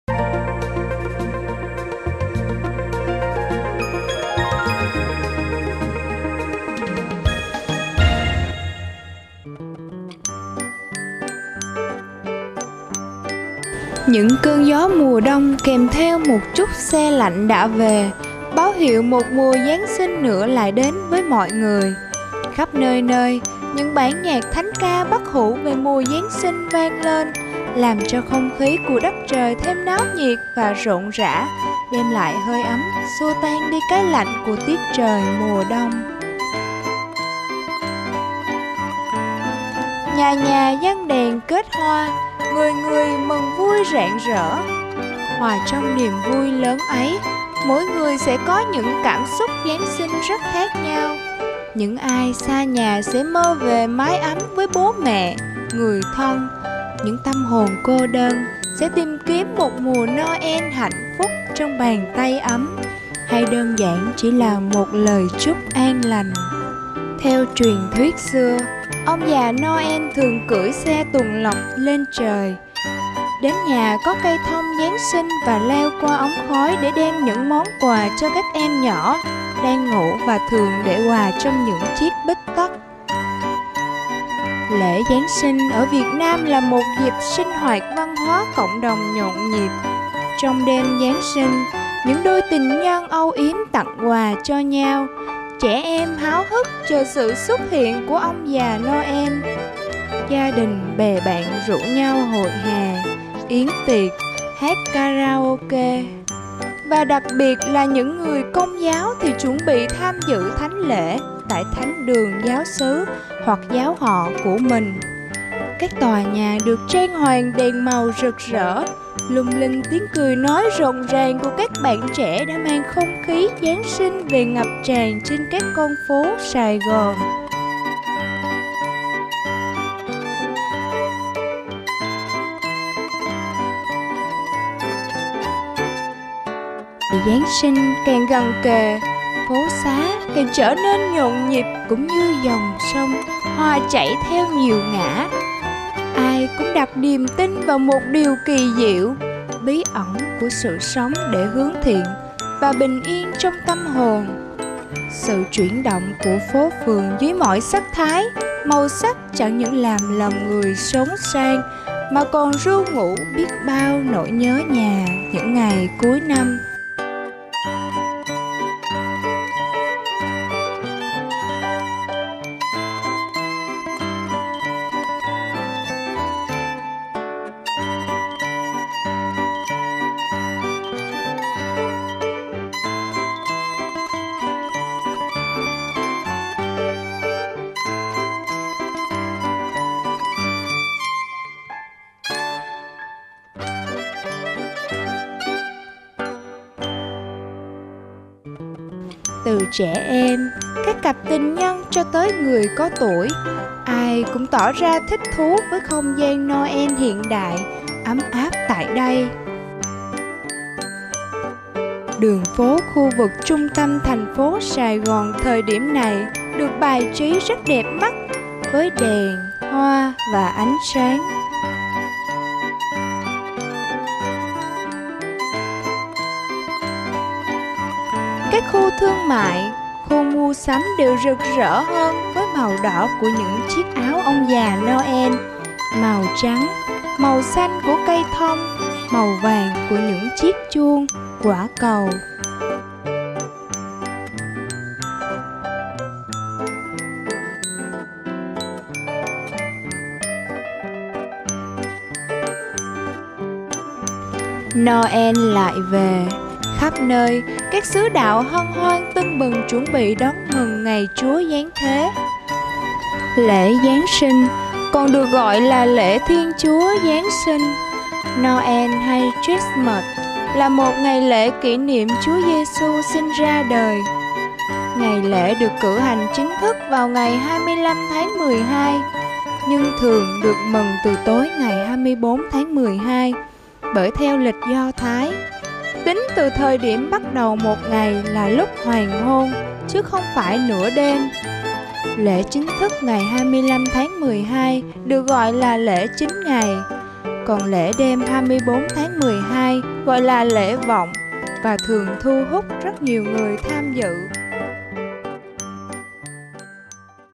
Sài Gòn tưng bừng đón Giáng Sinh
Noel lại về ! Khắp nơi,Đường phố khu vực trung tâm Sài Gònđược bài trí đẹp mắt với đèn, hoa và ánh sáng. Các khu thương mại, khu mua sắm, các tòa nhà được trang hoàng đèn màu, lung linh, rực rỡ hơn với màu đỏ của những chiếc áo ông già Noel.đã mang không khí Giáng Sinh về ngập tràn các con phố Sài gòn.